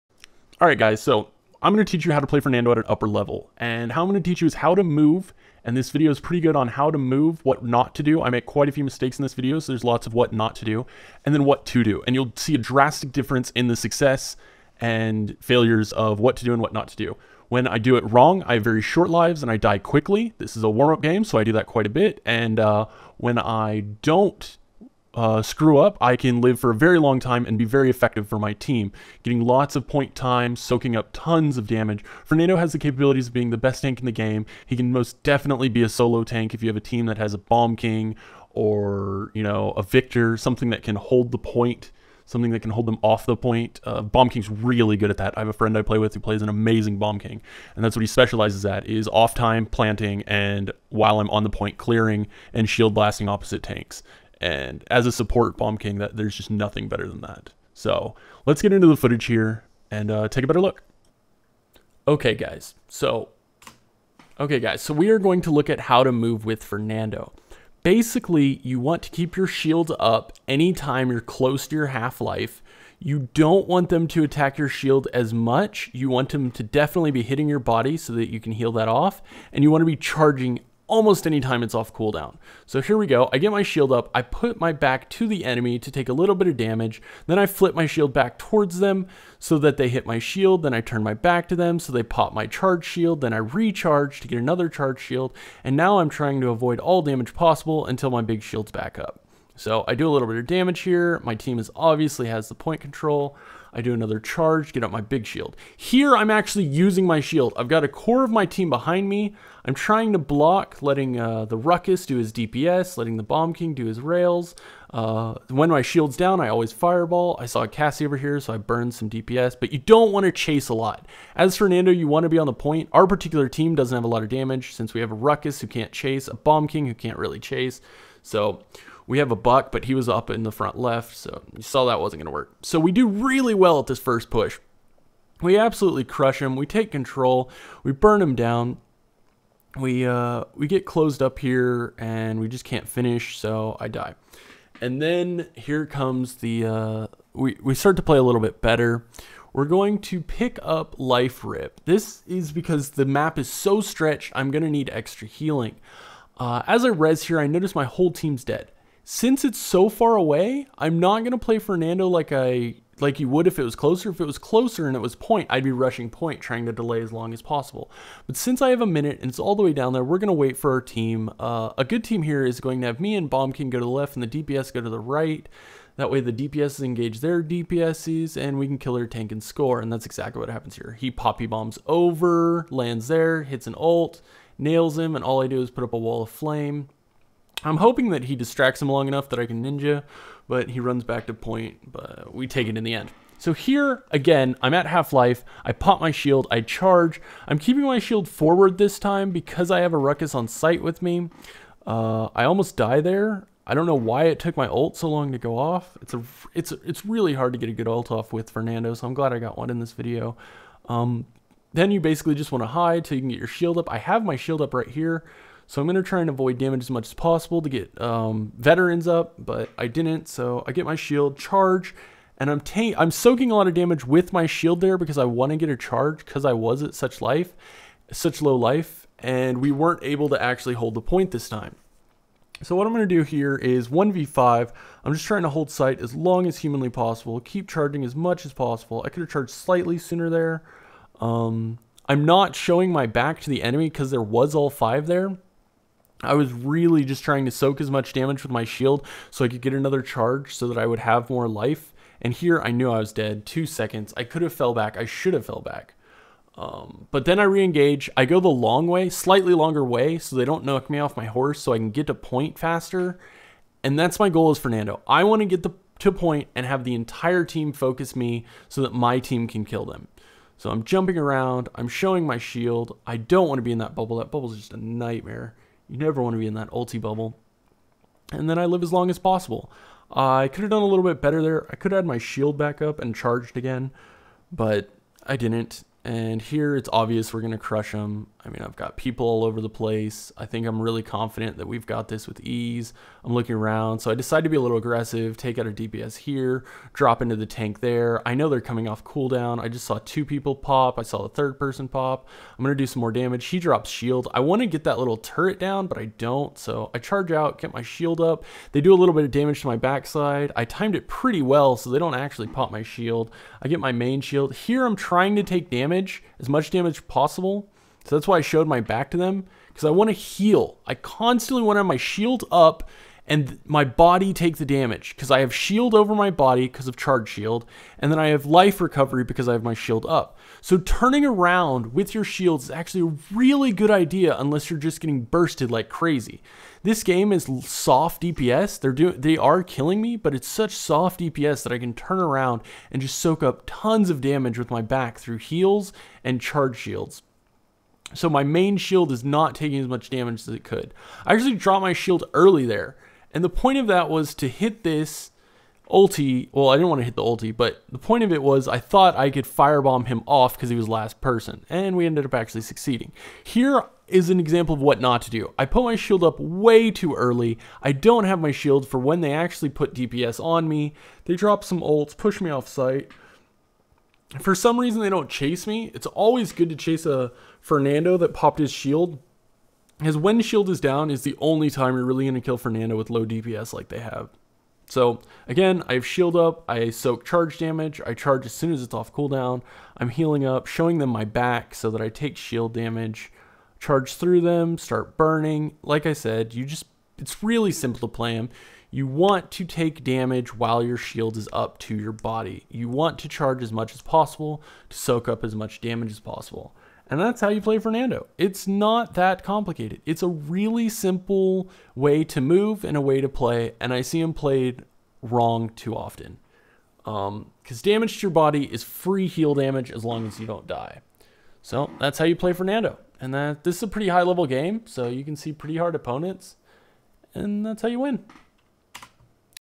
Fernando Movement and How to play top 100
i am crude and this video is pro, bad production quality no into no flash just content .just trying to make noob in to pros.